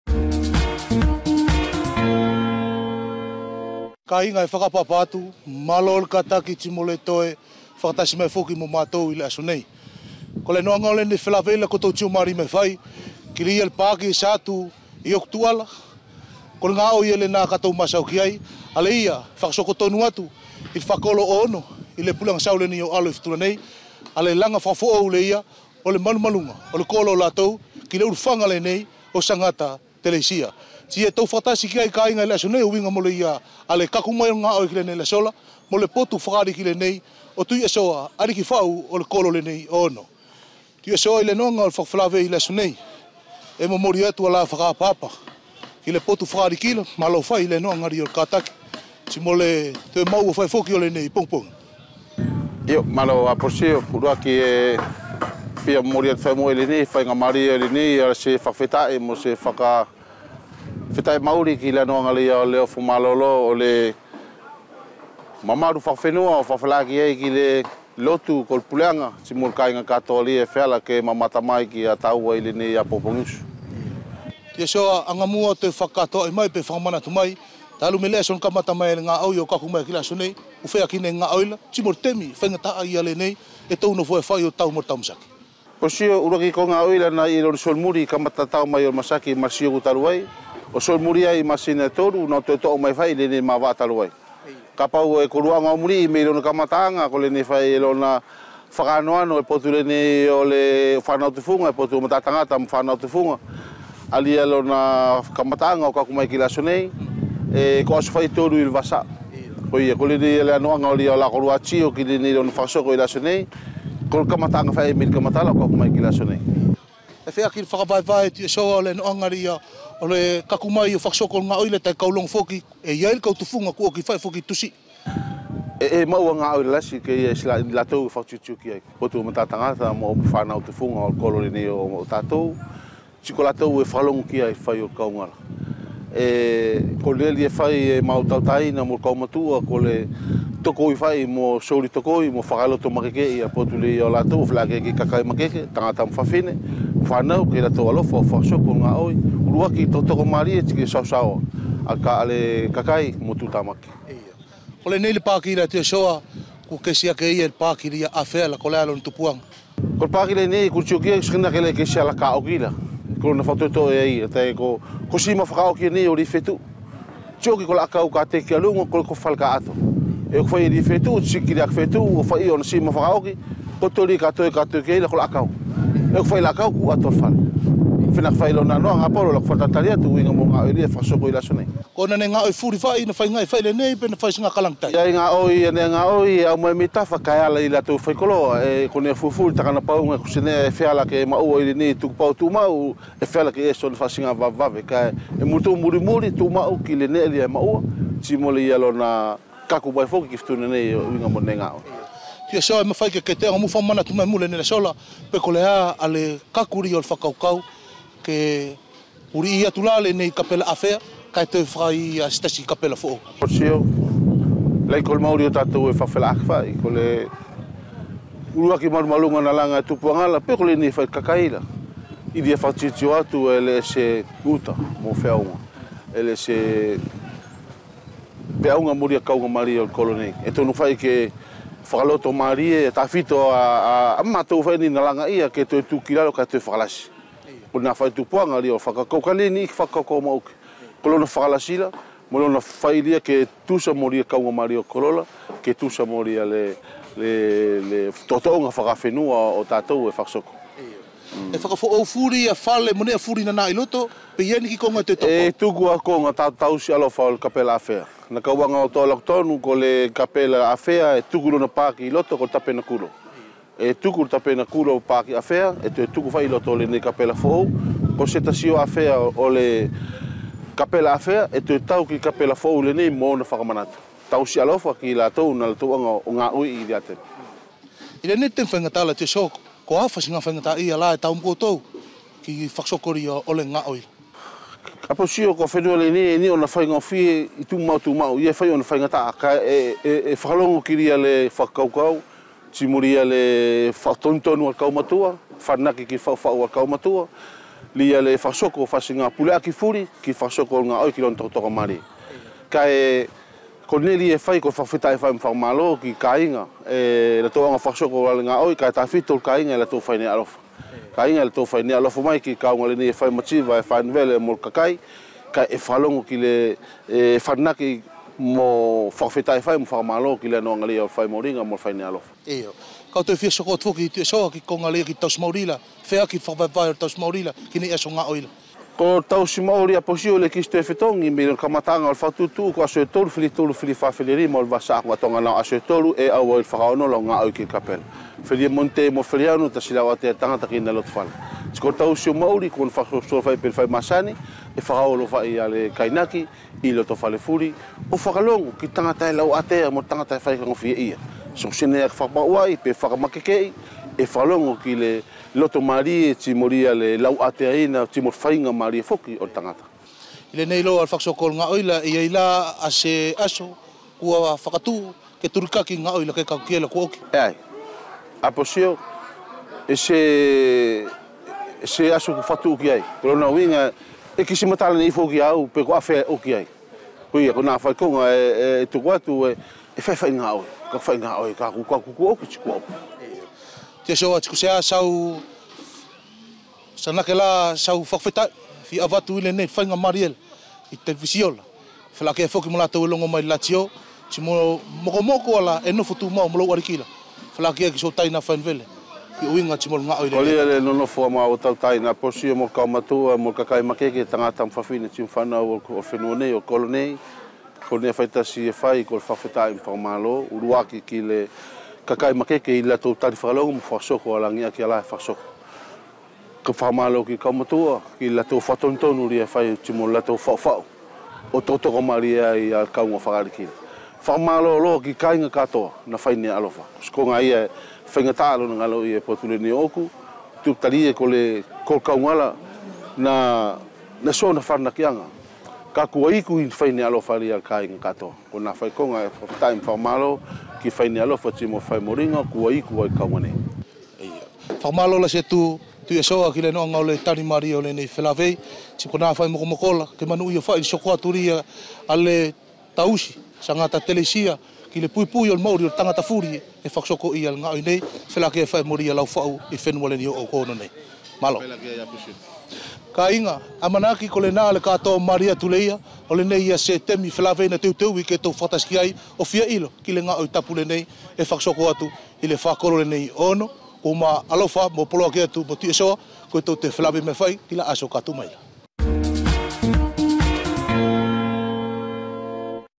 TALANOA: Rubrique Futuna - Sosefo Motuku
Apolosio Titilaiki fait le point sur le chantier actuellement en cours à la chapelle de Ono dans le royaume de Alo. Les explications du chef de village, Tu'iasoa Sosefo Motuku.